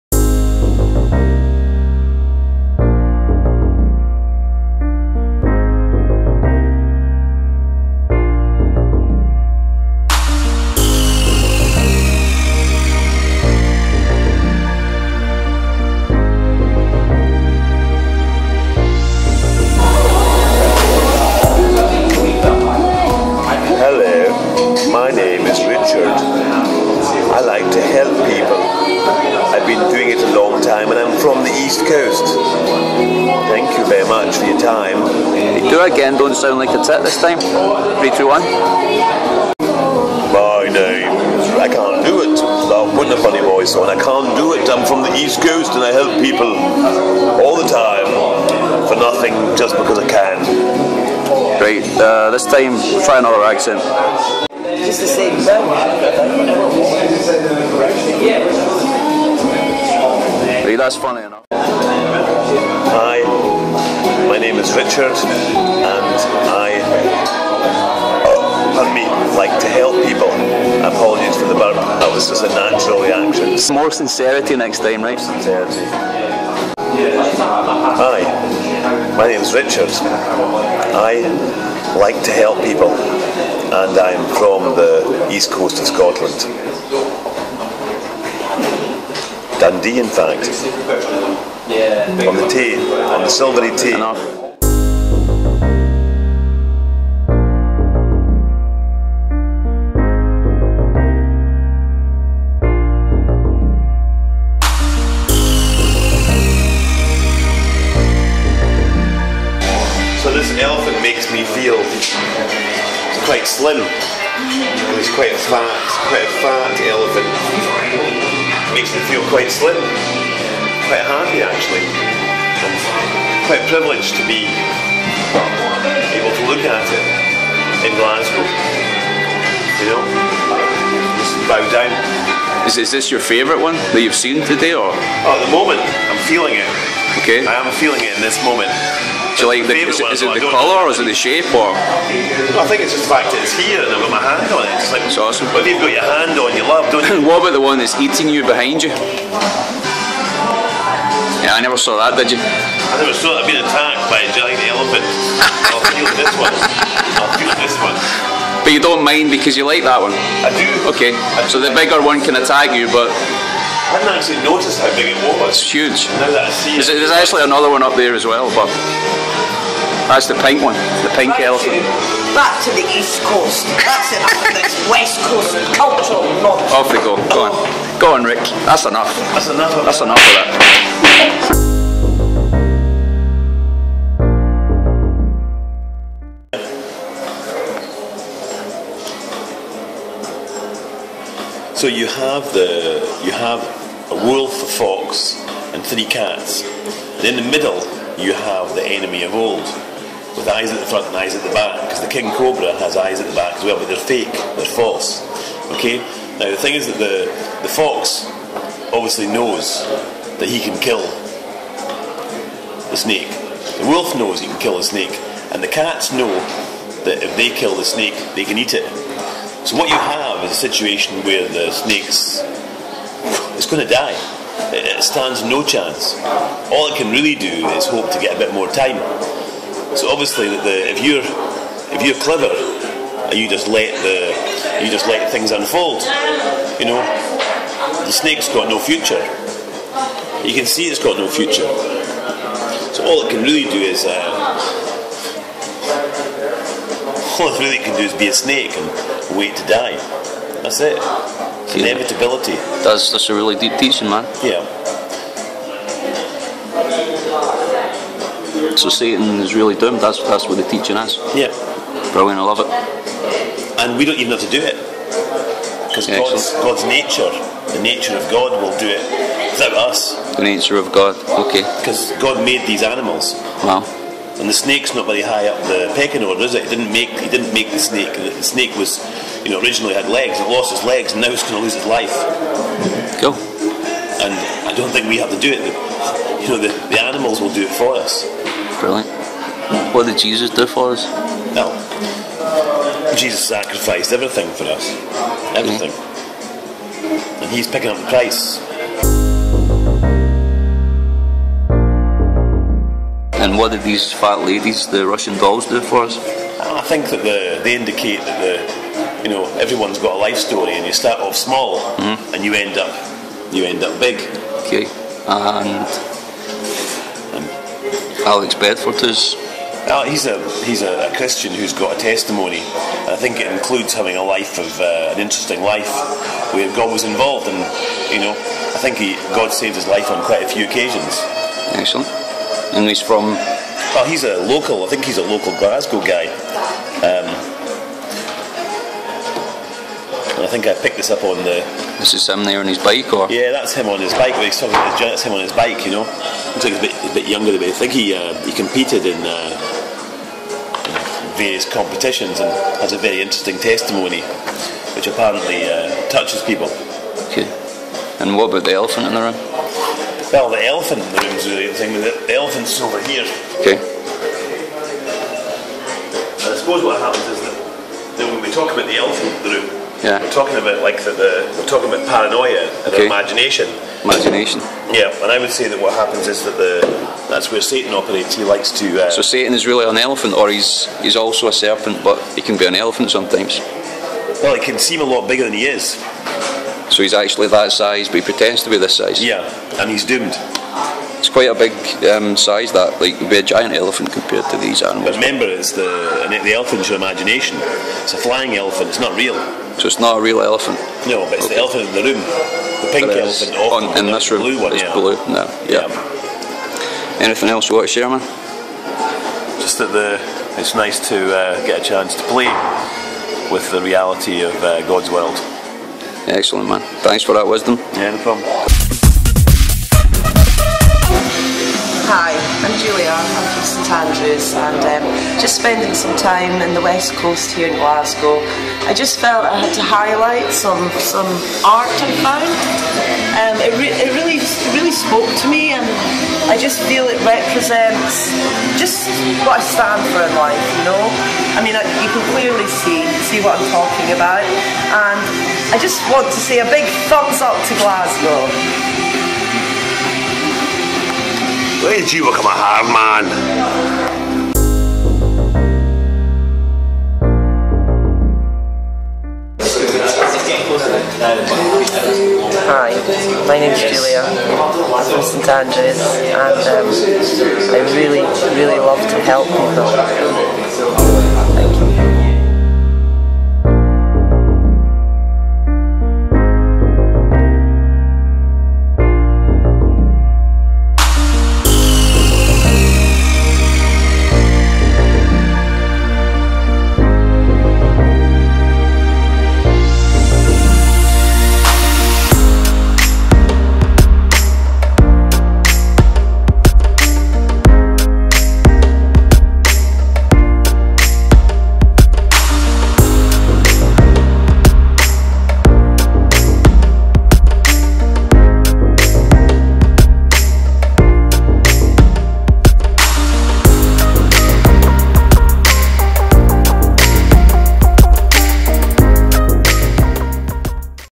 The Elephant in the Room....
Mr Tooti discovers an interesting guest at his birthday party.